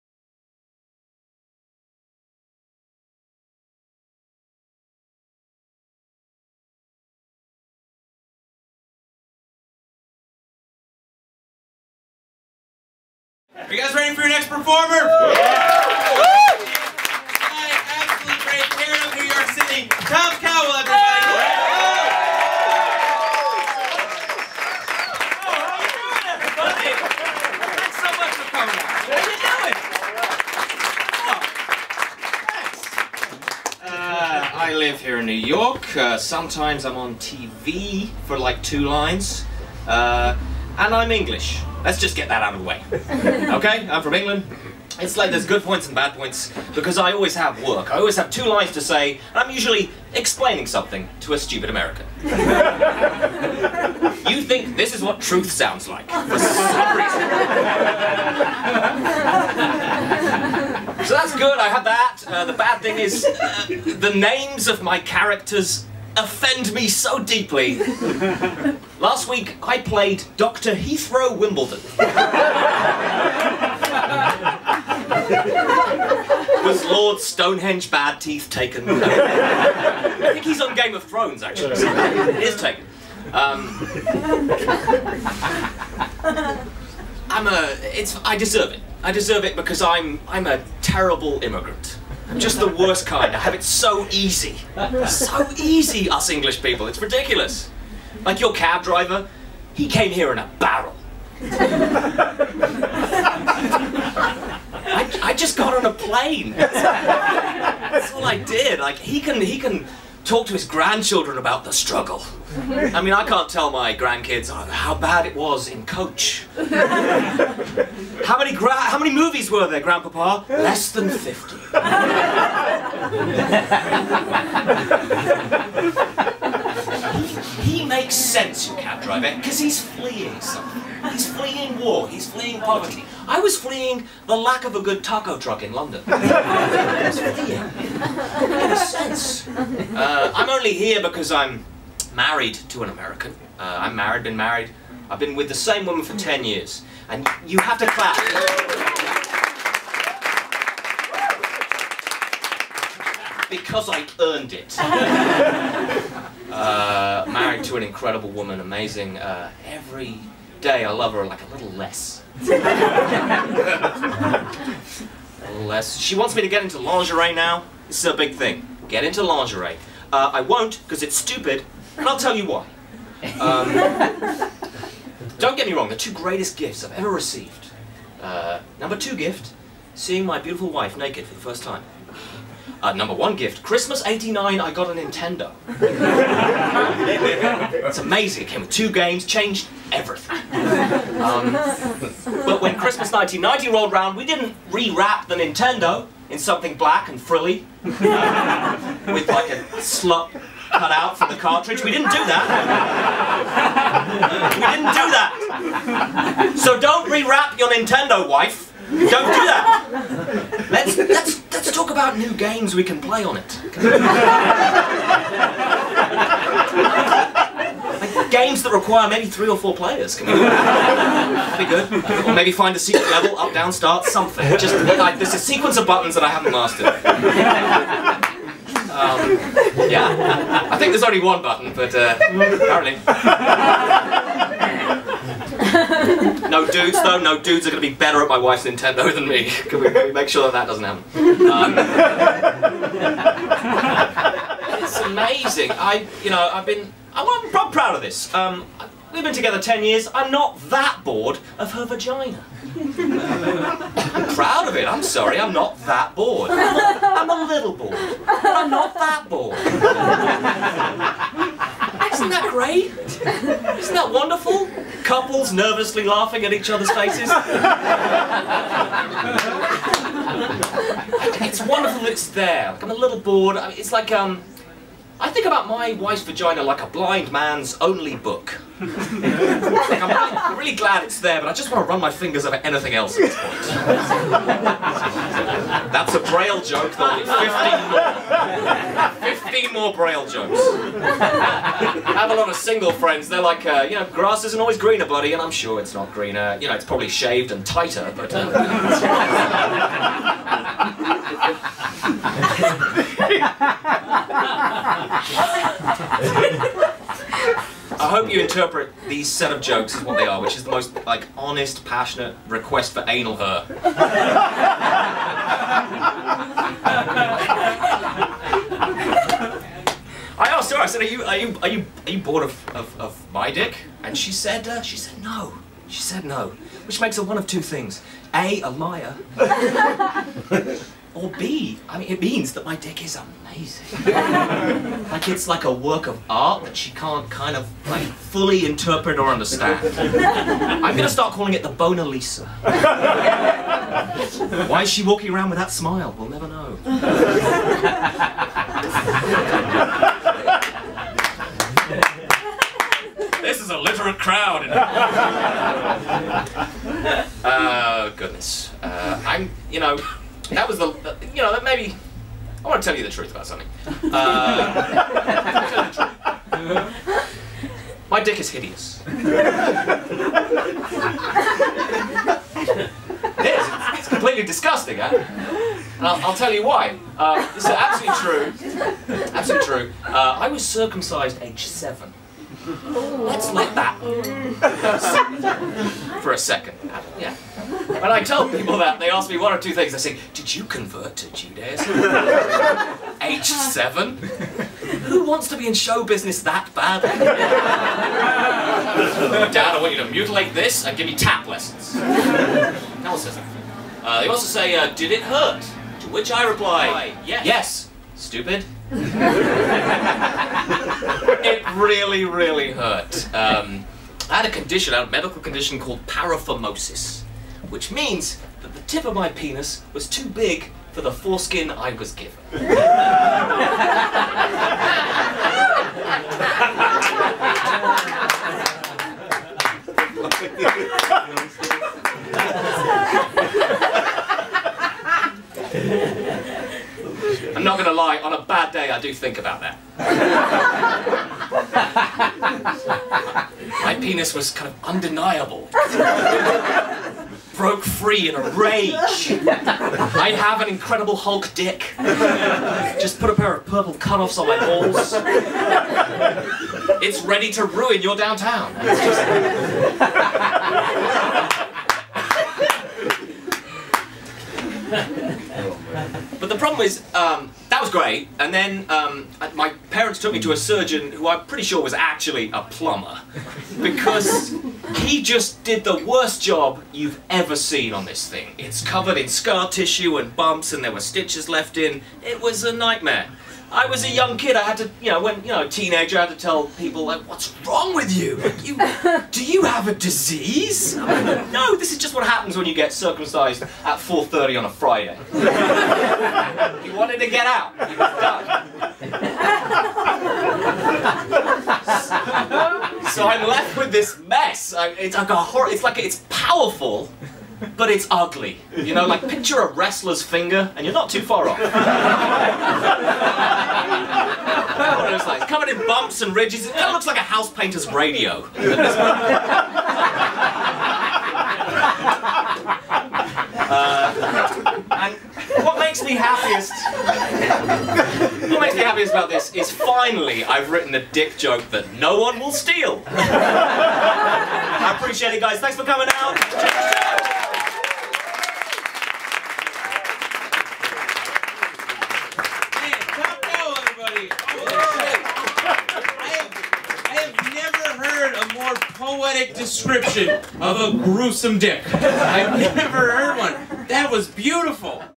Mr. Tom Cowell, stand-up comedy set
Taped at Legion, Brooklyn NY (2013)